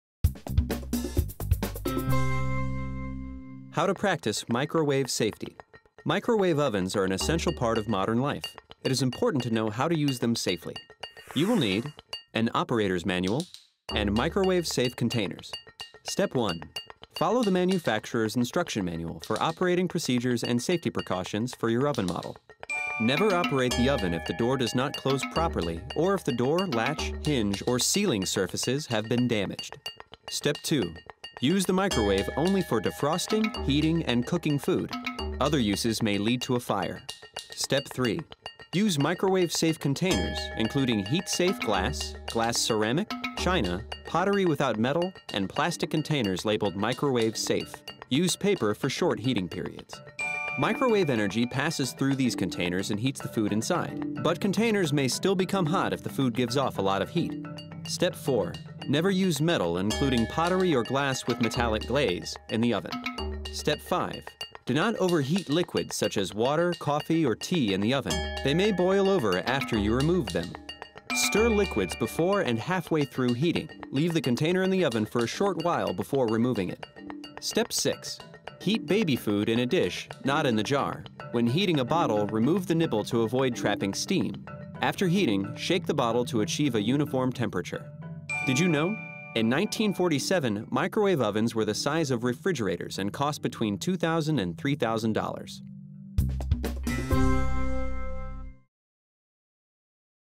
How to Practice Microwave Safety
Microwave ovens are an essential part of modern life. It is important to know how to use them safely.

Step 1: Follow the manufacturer’s instructions
Follow the manufacturer's instruction manual for operating procedures and safety precautions for your oven model.

Tip
Never operate the oven if the door does not close properly, or if the door, latch, hinge, or sealing surfaces have been damaged.

Step 2: Use the microwave oven appropriately
Use the microwave only for defrosting, heating, and cooking food. Other uses may lead to a fire.

Step 3: Use microwave-safe containers
Use microwave-safe containers including heat-safe glass, glass-ceramic, china, pottery without metal, and plastic containers labeled "Microwave-safe." Use paper for short heating periods.

Tip
Microwave energy passes through these containers and heats the food inside. But containers may still become hot if the food gives off a lot of heat.

Step 4: Avoid containers with metal
Never use metal, including pottery or glass with metallic glaze, in the oven.

Step 5: Do not overheat liquids
Do not overheat liquids such as water, coffee or tea in the oven. They may boil over after you remove them.

Tip
Stir liquids before and halfway through heating. Leave the container in the oven for a short while before removing it.

Step 6: Exercise caution with baby foods
Heat baby food in a dish, not in the jar. When heating a bottle, remove the nipple to avoid trapping steam. After heating, shake the bottle to achieve a uniform temperature.

Did you know? In 1947, microwave ovens were the size of refrigerators and cost between $2,000 and $3,000.